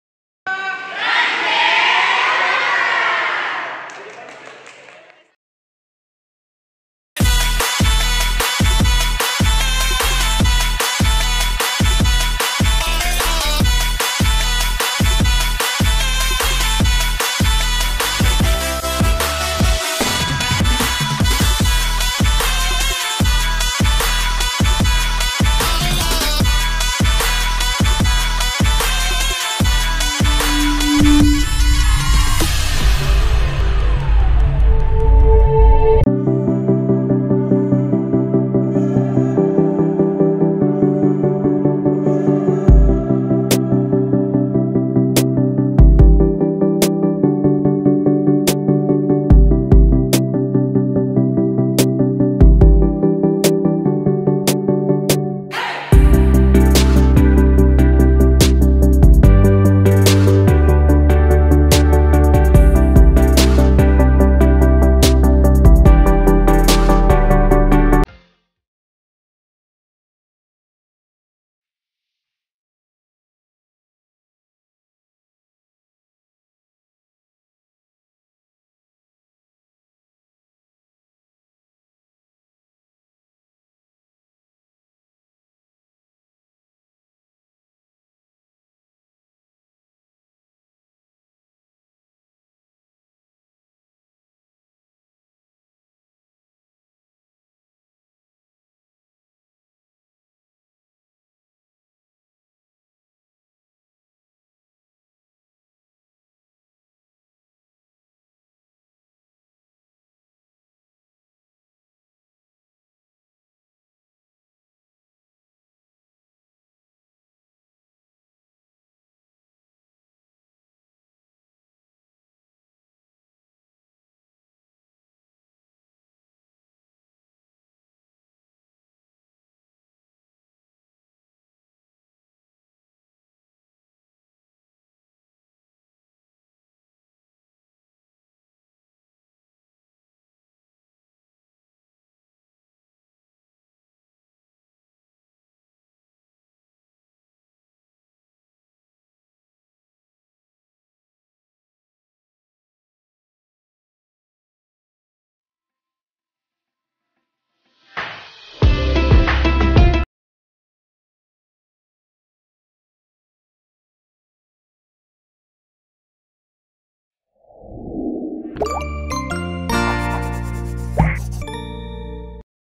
Bersama Berbagi Kebahagian - SMP Islam Athirah Kajaolaliddo
Rangkaian kegiatan Milad Sekolah Islam Athirah dengan tema "Bersama Berbagi Kebahagian"